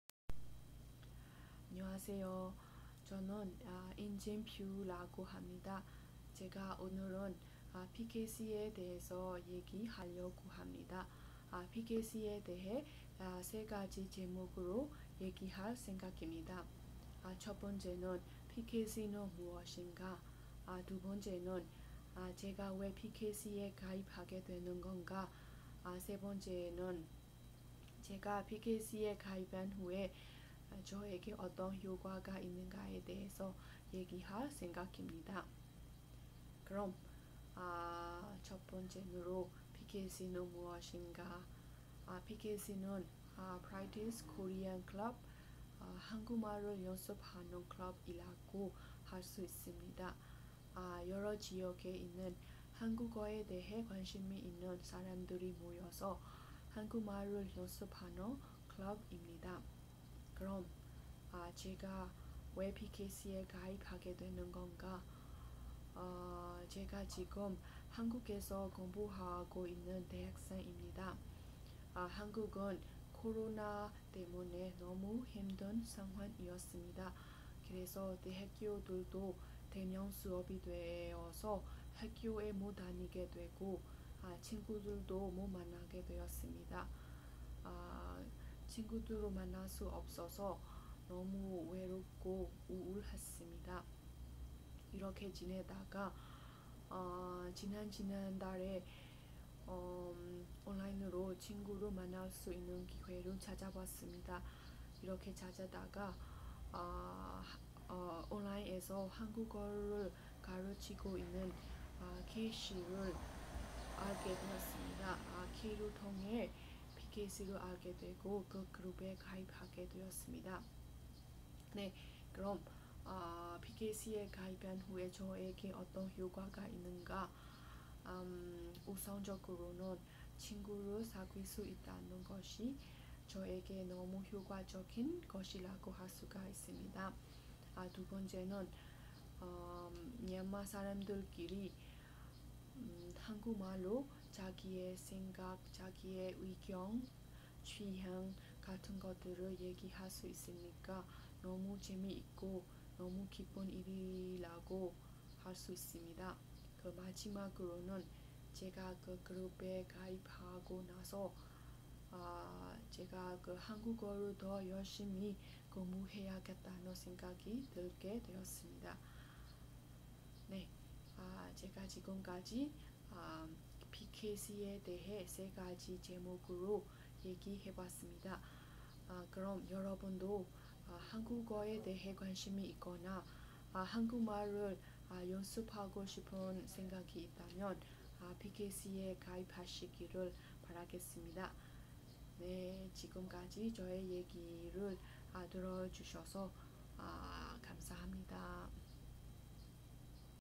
Inkyin's Feedback
Practice Korean Club

Member
Inkyin Phyu
PKC ကို Join စဉ်က ကိုရီးယားနိုင်ငံ Ulsan University မှာ ကိုရီးယားမေဂျာနဲ့ ကျောင်းတက်နေရင်း
ကိုရိုနာကြောင့် အွန်လိုင်းကနေပဲ တက်ရတဲ့အခါ စကားပြောဖော် သူငယ်ချင်းတွေ လိုချင်လို့
ပါဝင်ဖြစ်ခဲ့တာပါ။

အမ အင်ကြင်း အနေနဲ့ရော ဘာတွေ တိုးတက်ပြောင်းလဲသွားလဲဆိုတာကို ကြည့်ကြည့်ရအောင်နော်